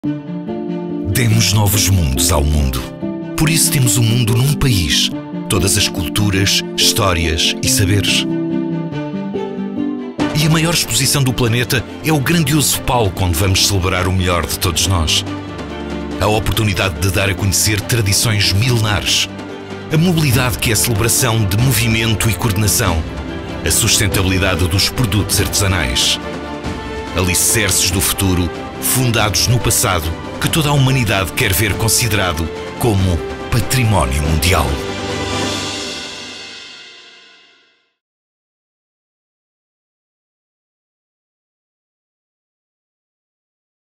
Porto and North of Portugal - Expo Dubai 2020
Presentation video for Expo Dubai 2020 participation.